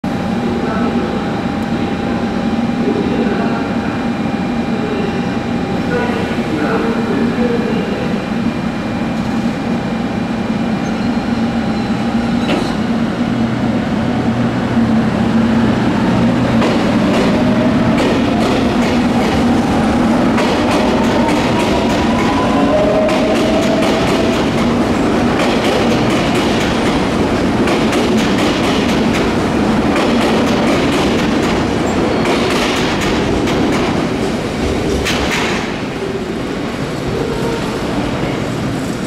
【4K】JR鹿児島本線 特急ソニック883系電車 小倉駅発車
鹿児島本線を走る特急列車です
特急ソニックは主に博多から小倉を経由して大分まで走る特急列車です